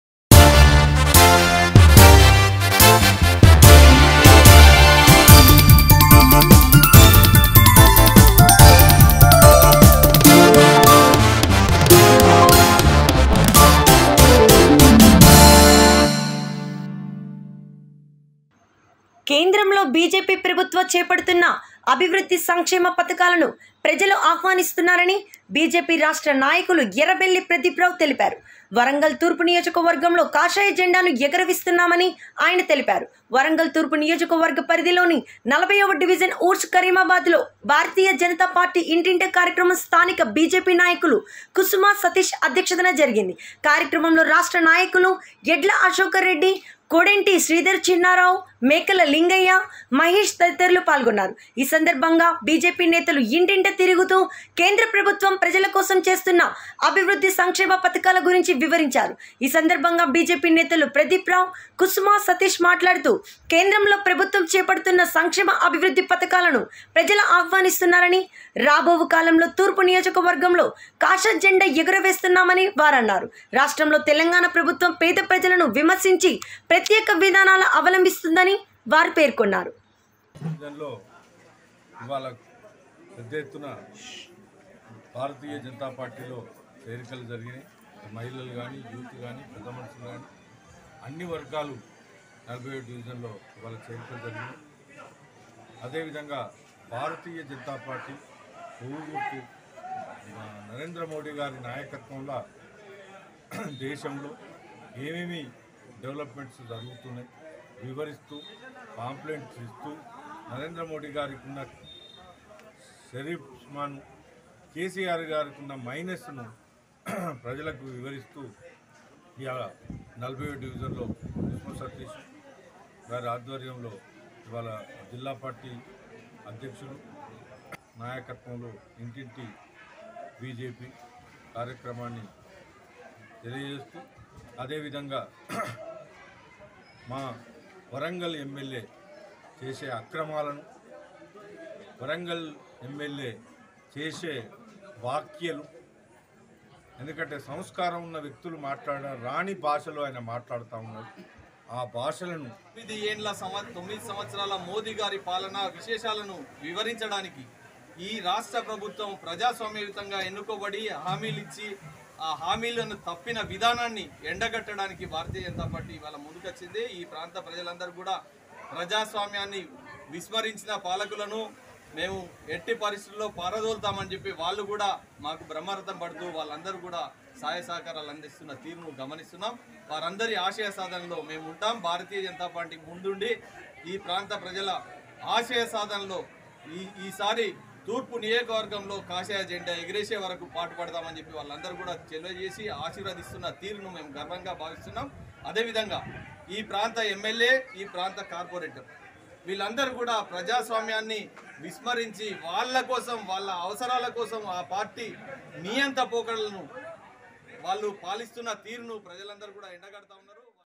BJP state leaders Errabelli Pradeep Rao development and welfare schemes BJP govt at the centre.
కేంద్రంలో బిజెపి ప్రభుత్వం చేపడుతున్న అభివృద్ధి సంక్షేమ పథకాలను ప్రజలు ఆహ్వానిస్తున్నారని బిజెపి రాష్ట్ర నాయకులు ఎర్రబెల్లి ప్రదీప్ రావు తెలిపారు లో వరంగల్ తూర్పు నియోజకవర్గంలో కాశాయ జెండాను ఎగురవేస్తామని ఆయన అన్నారు
వరంగల్ తూర్పు నియోజకవర్గ పరిధిలో నీ 40 వ డివిజన్ ఉర్సు కరీమాబాదులో భారతీయ జనతా పార్టీ ఇంటింటి కార్యక్రమం స్థానిక బిజెపి నాయకులు కుసుమ సతీష్ అధ్యక్షతన జరిగింది కార్యక్రమంలో రాష్ట్ర నాయకులు నాయకులు ఎడ్ల అశోక్ రెడ్డి కొండేటి శ్రీధర్ చిన్నారావు మేకల లింగయ్య మహేష్ తదితరులు పాల్గొన్నారు ఈ సందర్భంగా ఈ సందర్భంగా బిజెపి నేతలు ఇంటింటికి తిరుగుతూ కేంద్ర ప్రభుత్వం ప్రజల కోసం చేస్తున్న అభివృద్ధి సంక్షేమ పథకాల గురించి వివరించారు ఈ సందర్భంగా బీజేపీ నేతలు ప్రదీప్ రావు కుసుమ సతీష్ లు మాట్లాడుతూ కేంద్రంలో ప్రభుత్వం చేపడుతున్న సంక్షేమ అభివృద్ధి పథకాలను ప్రజల ఆహ్వానిస్తున్నారని రాబోవు కాలంలో తూర్పు నియోజకవర్గంలో కాషా జండా ఎగరవేస్తామని వారన్నారు రాష్ట్రంలో తెలంగాణ ప్రభుత్వం పేద ప్రజలను విస్మరించి ప్రత్యేక విధానాల అవలంబిస్తుందని వారు పేర్కొన్నారు
BJP state leaders Errabelli Pradeep Rao said that the people are welcoming the development and welfare schemes being implemented by the BJP government at the centre.